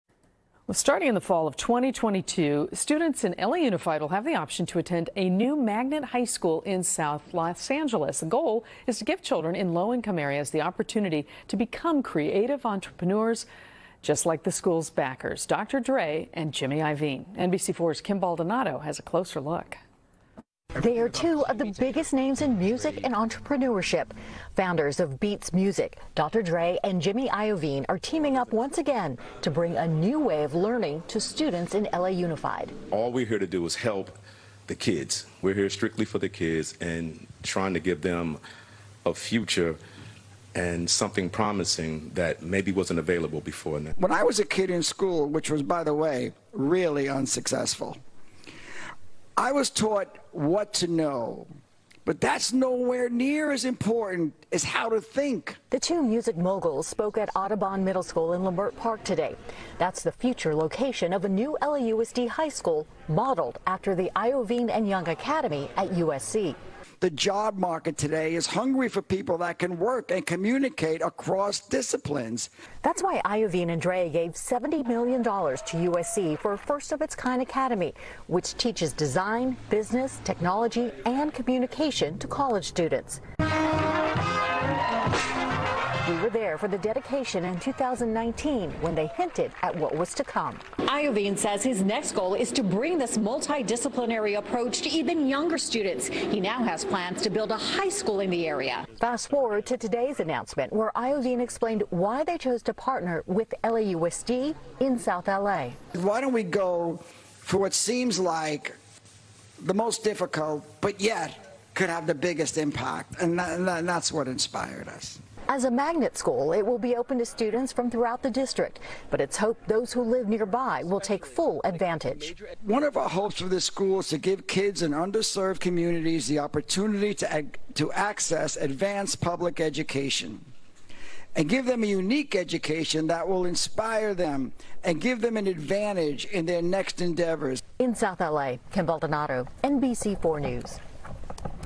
Music Moguls Dr. Dre, Jimmy Iovine, Launch School for Entrepreneurs | NBCLA
Students in LA Unified will get a chance to attend a new magnet school in South LA. Kim Baldonado reports for the NBC4 News on Monday, June 14, 2021.

About NBCLA